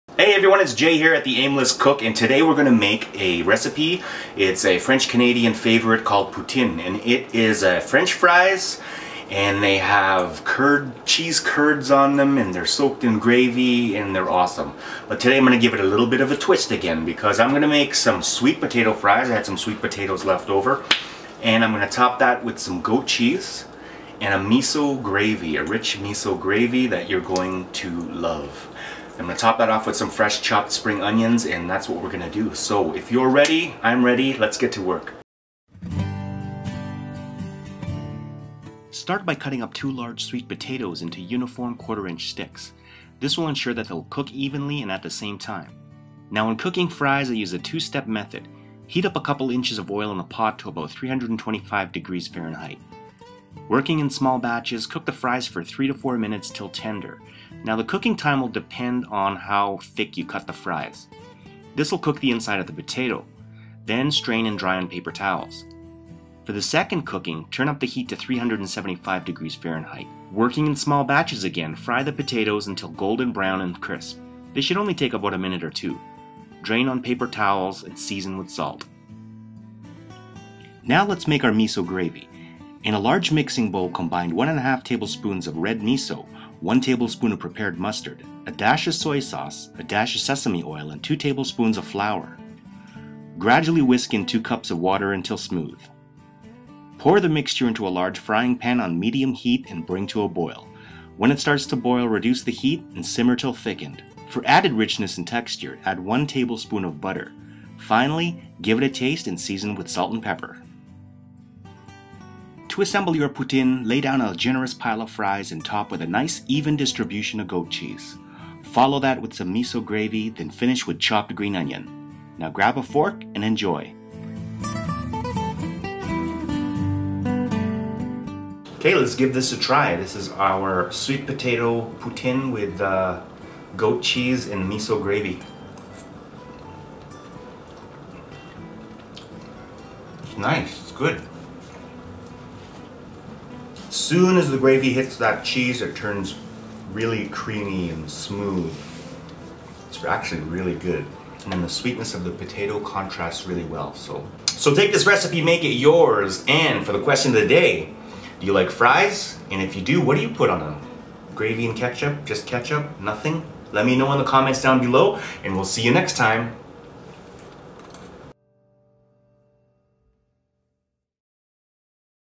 Sweet Potato Poutine Recipe - Canadian Favourites
Poutine is a French Canadian favourite that is enjoyed all over Canada. Traditionally, it's comprised of French fries, cheese curds and a rich gravy, although there are many variations and toppings available today such as smoked meat, bbq, or pulled pork to name a few. Today, I am making my poutine with sweet potato fries and topping them with fresh goat cheese and a rich butter miso gravy. The goat cheese goes well with the sweet potato and is all brought together with the savory flavor of the miso gravy. I hope you try this at home and enjoy!